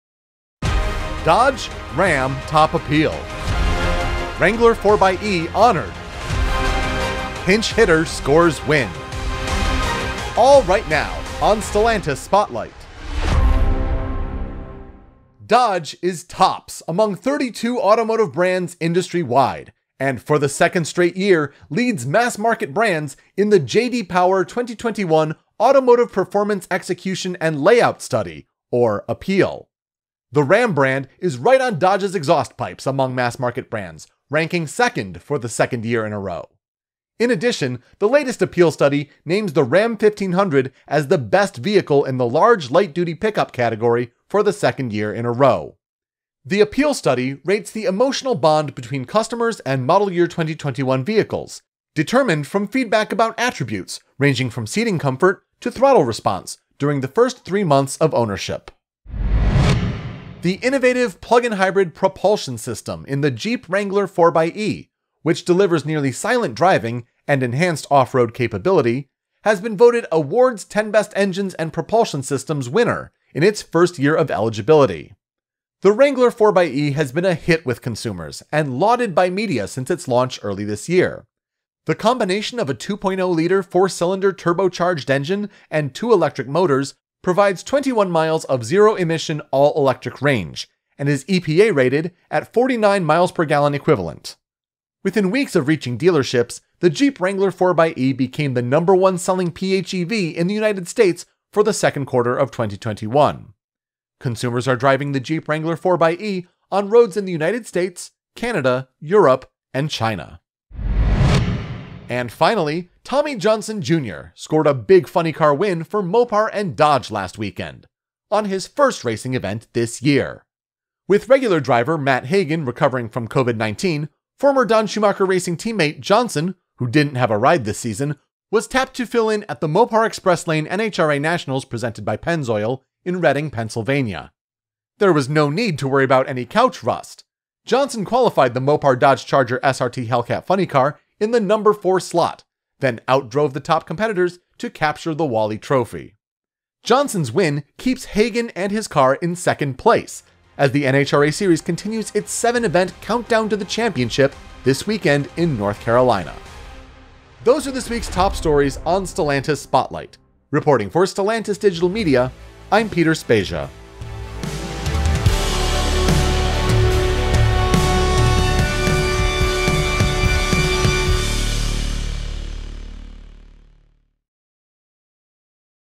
Stellantis Spotlight: September 17, 2021
"Stellantis Spotlight" is a weekly recap of some of the major stories at Stellantis. The top stories for the week ending September 17, 2021, include top rankings for the Dodge and Ram brands in the J.D. Power APEAL Study; the Jeep® Wrangler 4xe’s plug-in hybrid propulsion system is named to the Wards 10 Best list; and substitute driver Tommy Johnson Jr. scores big for Mopar and Dodge at the Mopar Express Lane NHRA Nationals Presented by Pennzoil.